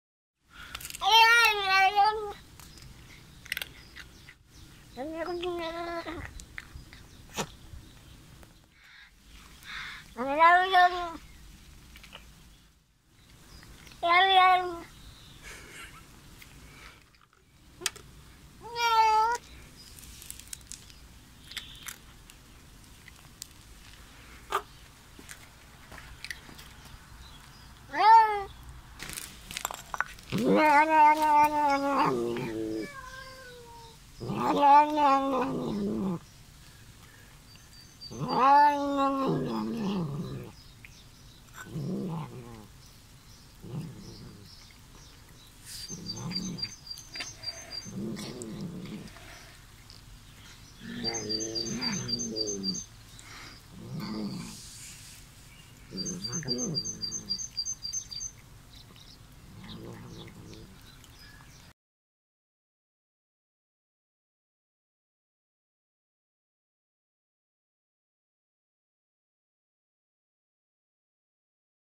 Loud Bengal Kitten eating with funny Sounds - ( Nom Nom Cat )
A foreign bengal kitten visits us and makes funny sounds while eating. (om nom nom ) ...   Hes a loud one but so adorable and sweet at the same time :)